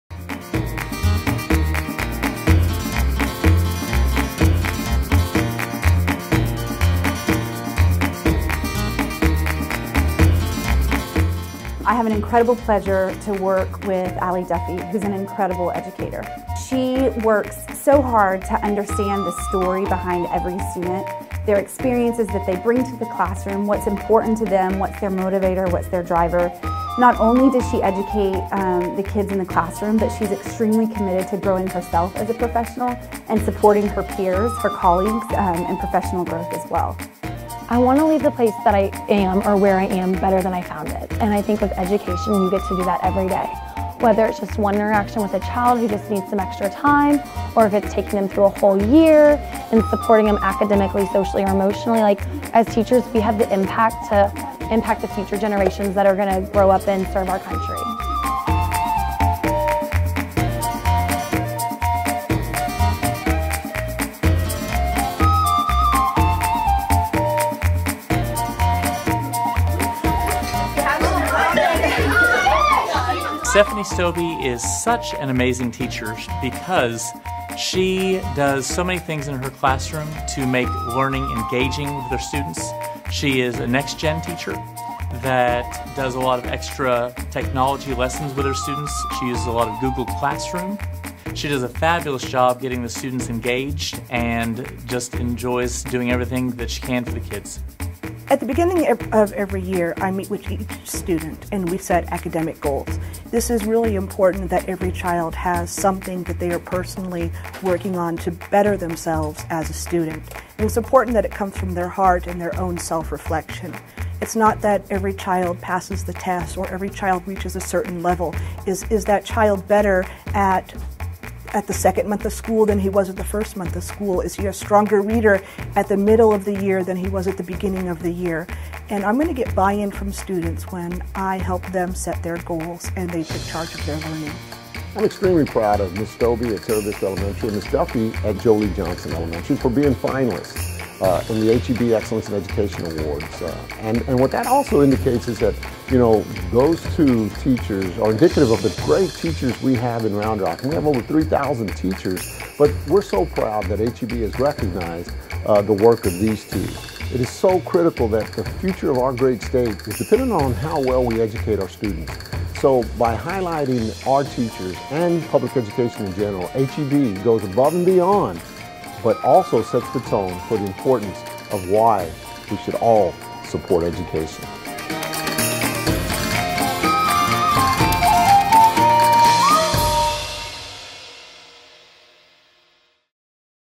Joe Lee Johnson, Teravista teachers finalists for H-E-B Excellence in Education awards
Joe Lee Johnson Elementary STEAM Academy teacher Allie Duffy and Teravista Elementary School teacher Stephanie Stoebe were named finalists for H-E-B Excellence in Education. They earned $1,000 for both their classrooms and their schools.